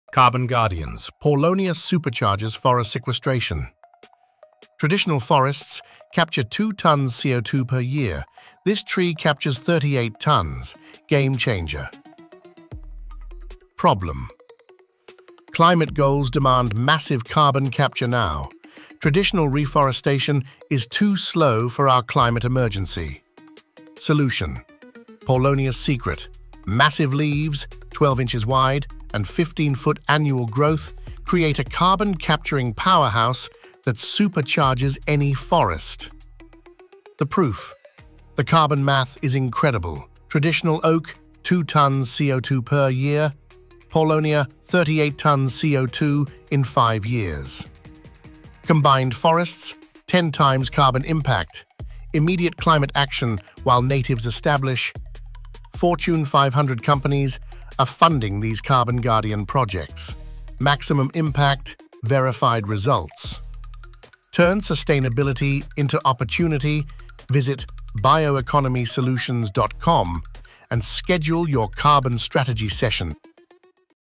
Paulownia the Future of Carbon Capture
👉 Interested in Growing Paulownia? A step-by-step guide with EVERYTHING you need to get started with Paulownia farming.

Did you know that fast-growing Paulownia trees are being planted worldwide for their eco-friendly benefits? From reforestation projects to sustainable timber, these "miracle trees" are making a difference. Discover how Paulownias are helping the planet—one tree at a time! 🌱✨

What You Will Learn:
Paulownia trees are renowned for their remarkable ability to address and even reverse several negative environmental factors.
This will show you some key ecological benefits paulownia trees provide:

- Soil Restoration & Erosion Control
- Carbon Sequestration (Fights Climate Change)
- Phytoremediation (Cleans Polluted Soil & Water)
- Biodiversity Support
- Reduces Deforestation Pressure
- Fast Growth
- Where to purchase non-invasive paulownia hybrid tree saplings

If you're interested in paulownia, want to grow or currently growing, this information is for you.

♻️ Repost this to help your network
👉 Follow @BioEconomySolutions for more

👉Paulownia trees absorb 10X more CO2 than oak. Want to monetize yours?